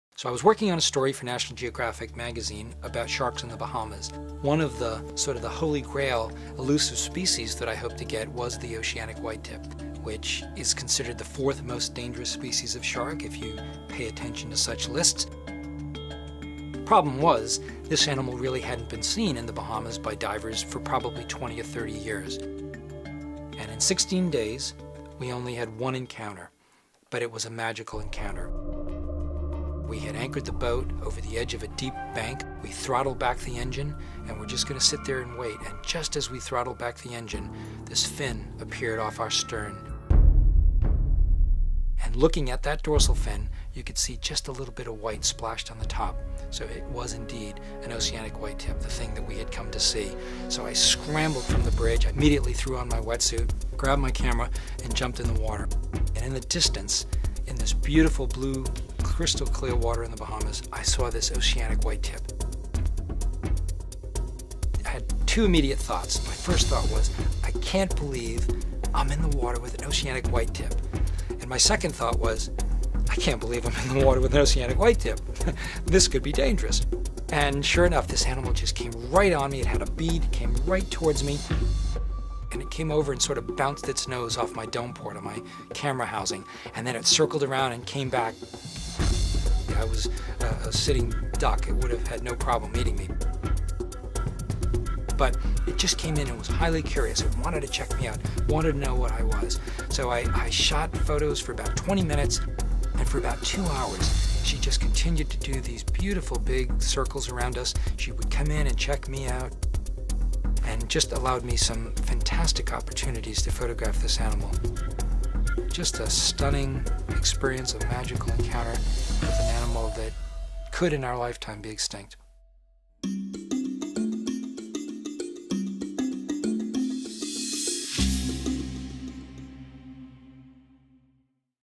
Ocean Soul: Close Encounters with a Dangerous Shark
National Geographic photographer Brian Skerry describes the incredible day he spent photographing a rare, beautiful, and dangerous creature: the Oceanic Whitetip shark. With his new book from National Geographic, "Ocean Soul," Brian showcases his stunning photography and describes his adventurous life in a gripping portrait of the ocean as a place of beauty and mystery, a place in trouble, and ultimately, a place of hope that will rebound with the proper attention and care.